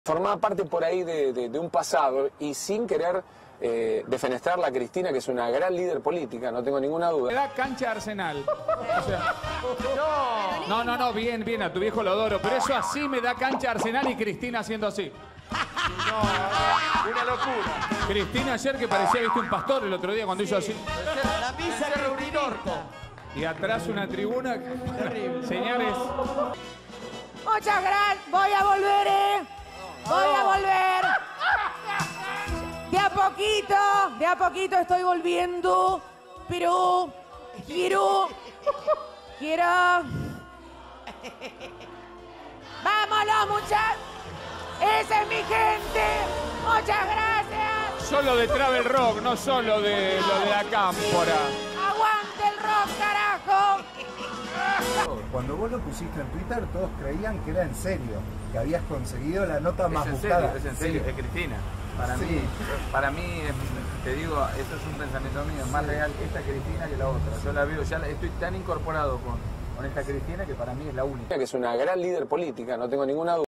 Tinelli no resiste el archivo
De anti kirchnerista a kirchnerista furioso. De criticar a Cristina a elogiarla.